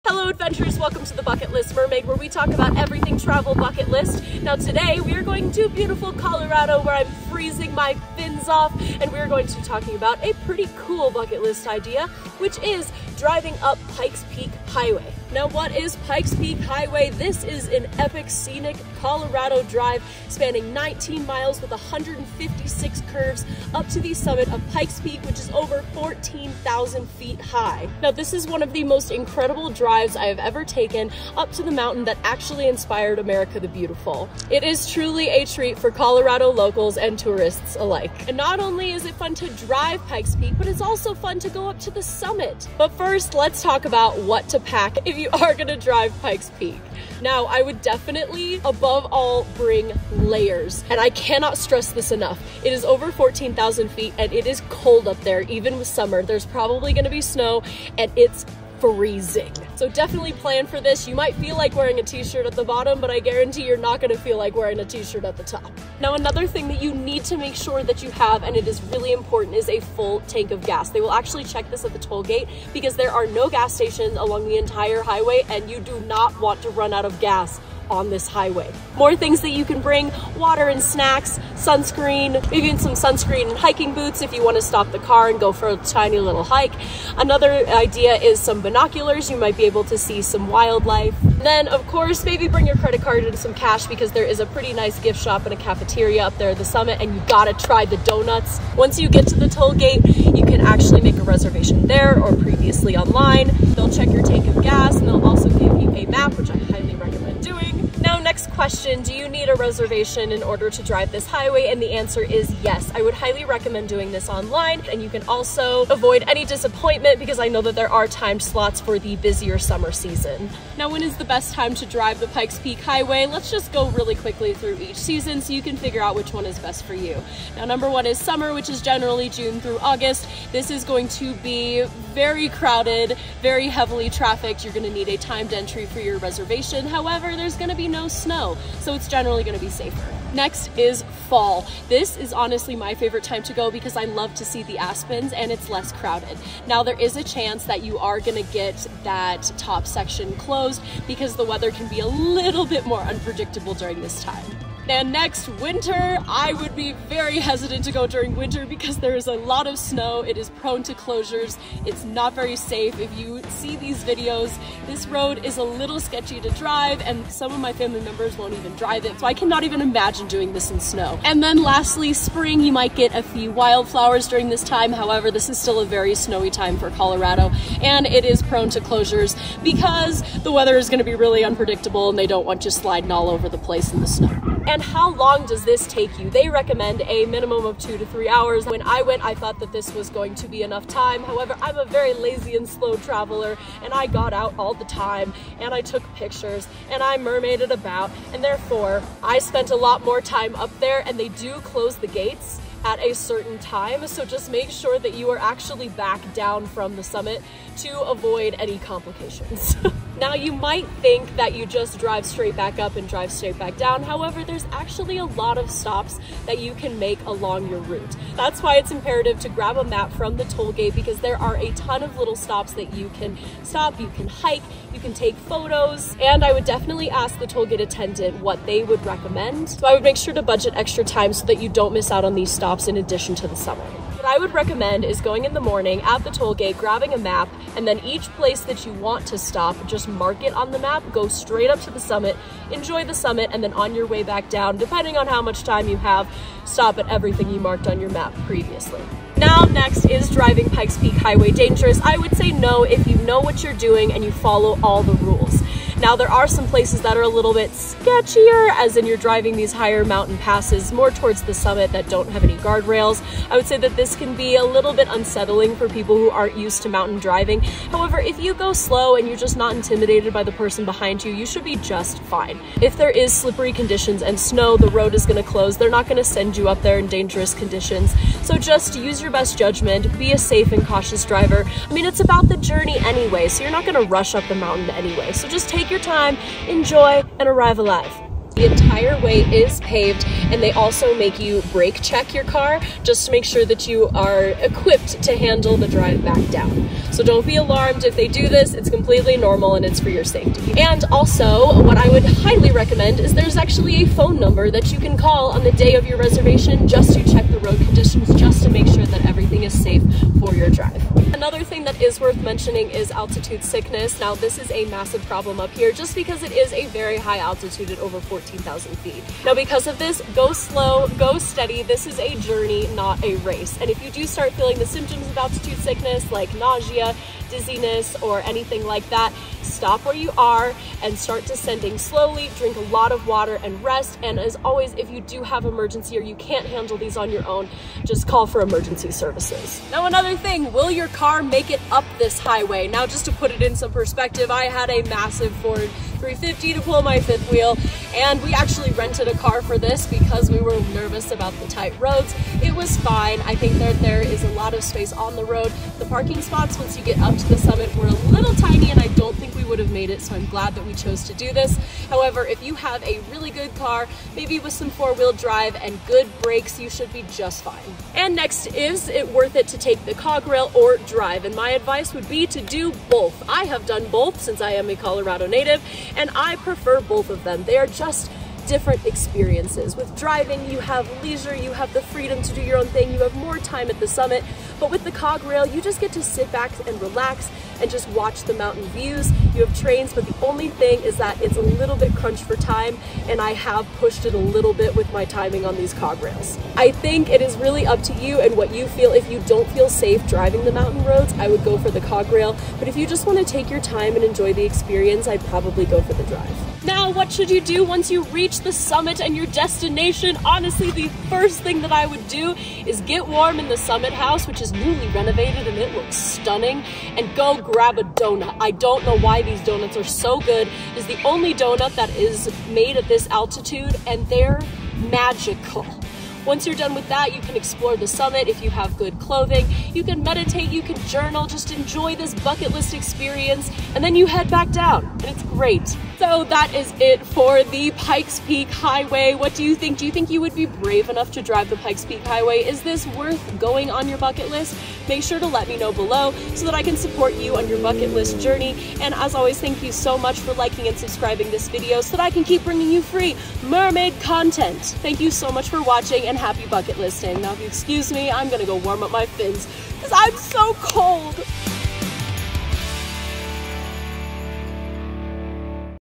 Driving Pikes Peak Highway (A Guide from a CO Local)
Welcome to Pikes Peak Highway in Colorado, a must-visit destination for any road trip enthusiast!

This iconic highway takes you on a thrilling journey to the summit of Pikes Peak, one of the most famous mountains in the United States.

In this video, I'll guide you as we drive through hairpin turns and breathtaking scenery. But if you want the full experience and all the insider tips, don't forget to check out our full article linked below:

Trust me, this is one bucket list adventure you won't want to miss. So buckle up and prepare for an unforgettable ride on Pikes Peak Highway in Colorado!